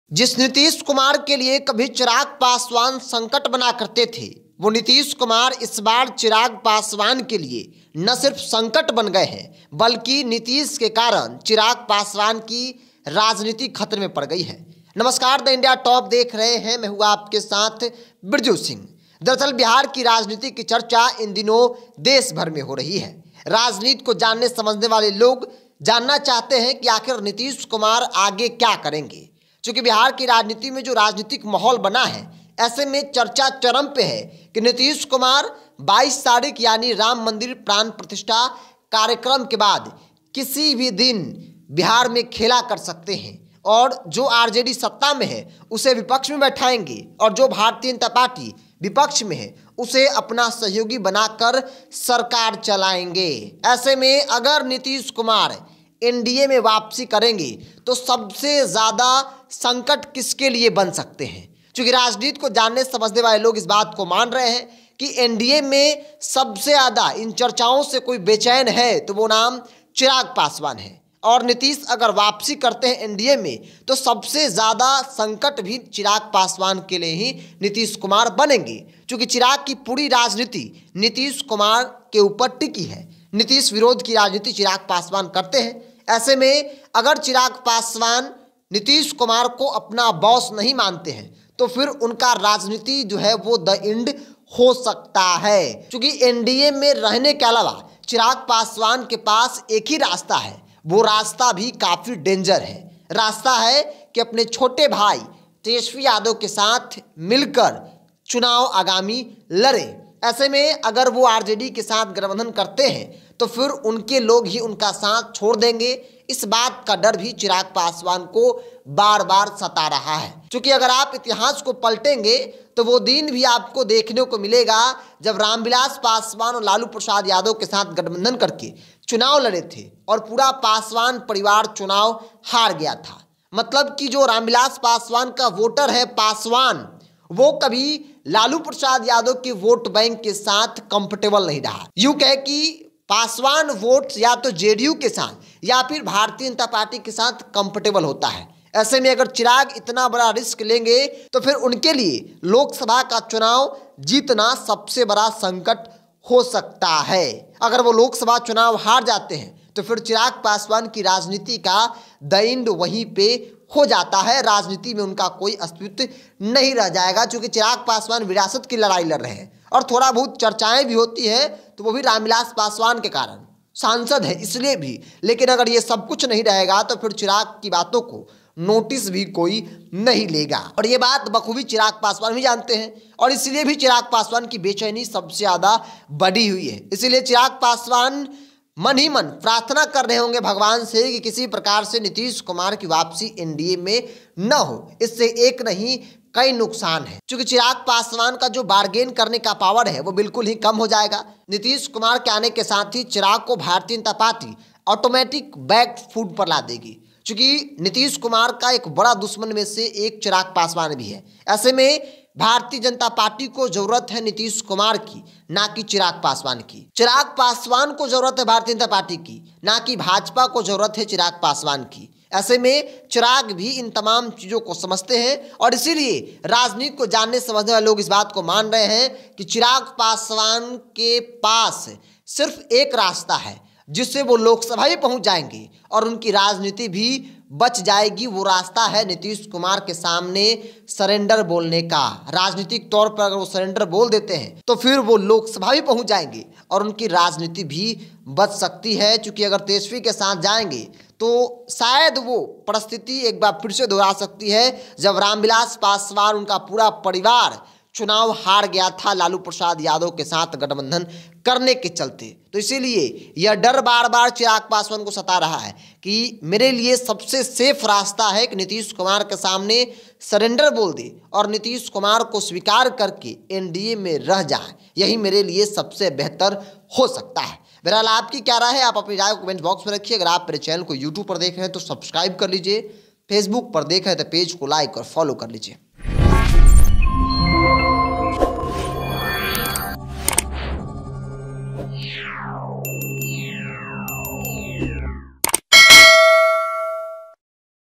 Chirag के लिए Nitish Kumar बने संकट! Tejashwi के साथ गए तो राजनीति खत्म! BJP भी नहीं दे रही भाव!
Chirag Paswan के लिए Nitish Kumar बने संकट! Tejashwi Yadav के साथ गए तो राजनीति खत्म! BJP भी नहीं दे रही भाव!